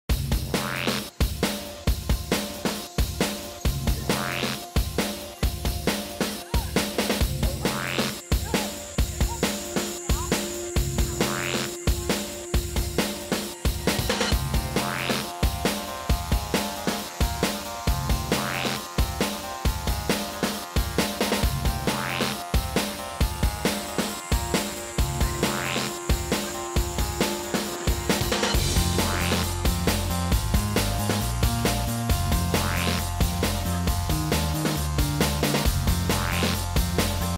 Grace Jonez and I run the agility course
Pretty good for only training about 5 months